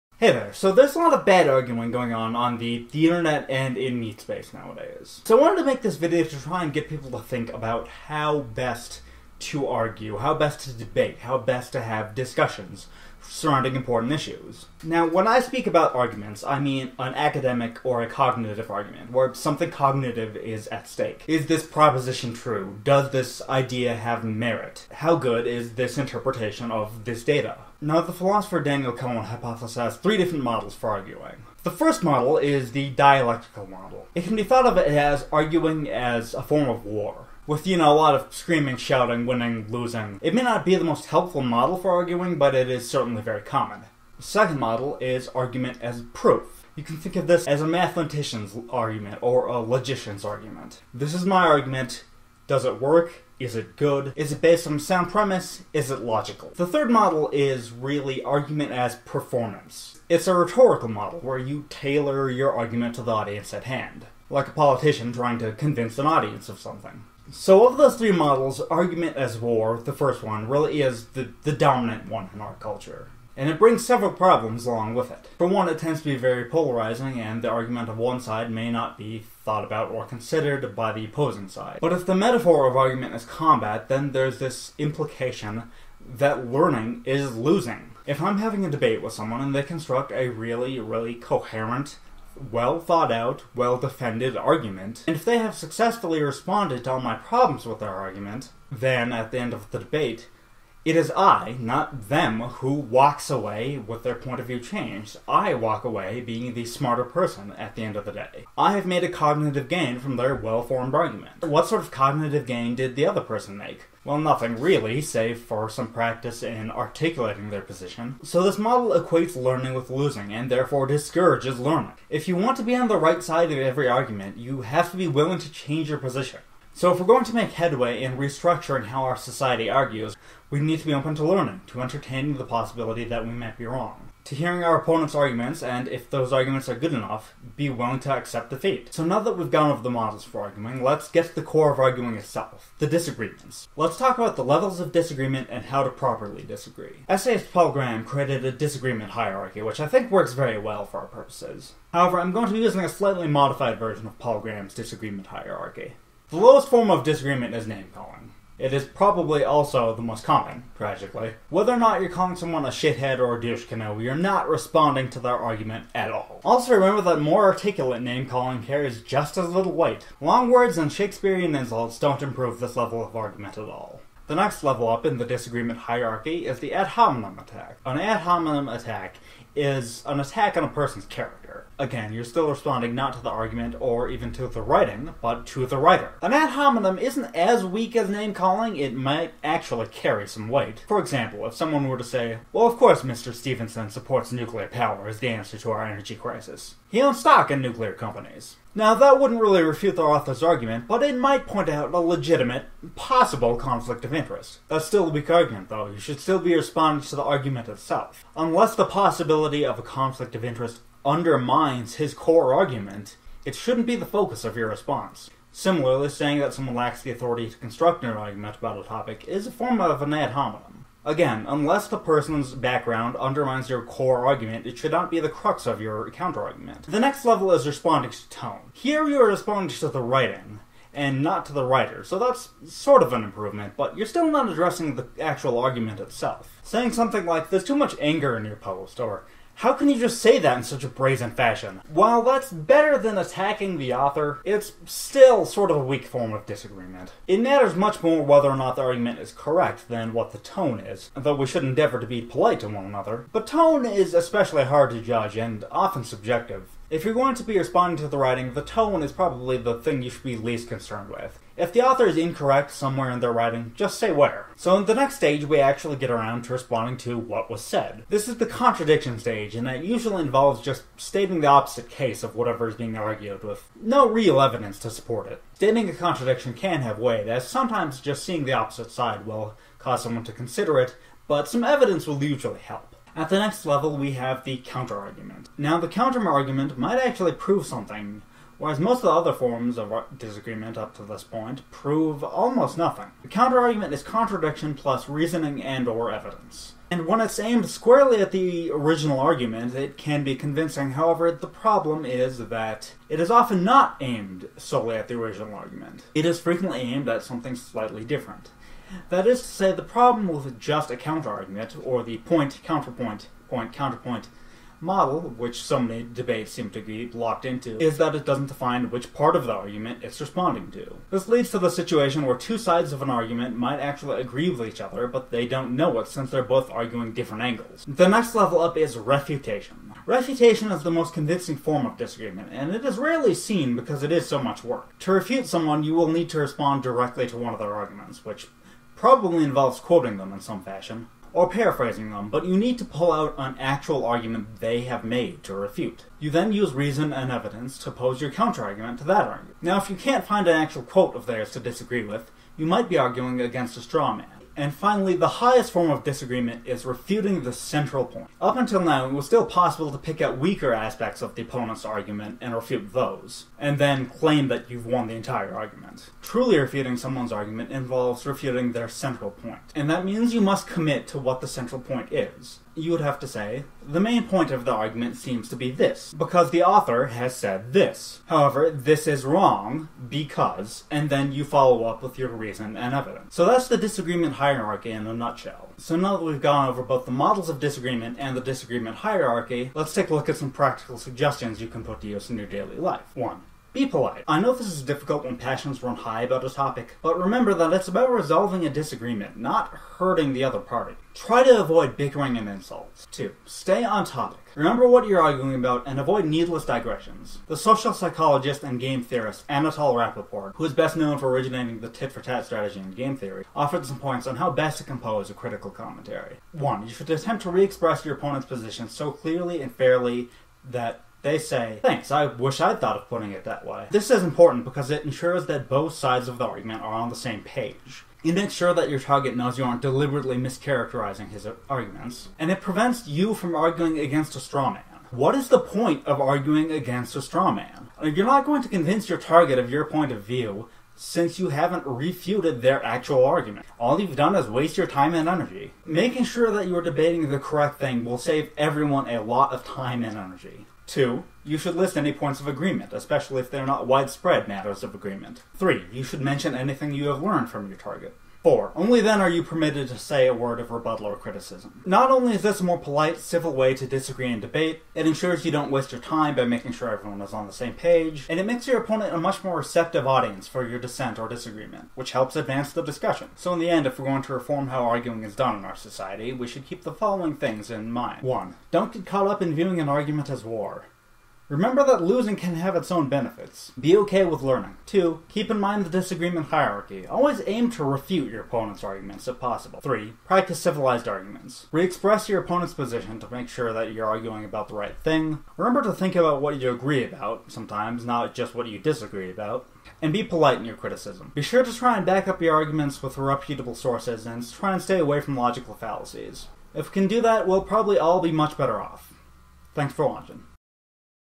How To Properly Argue
A guide on how to debate in a more effective, smarter, politer fashion.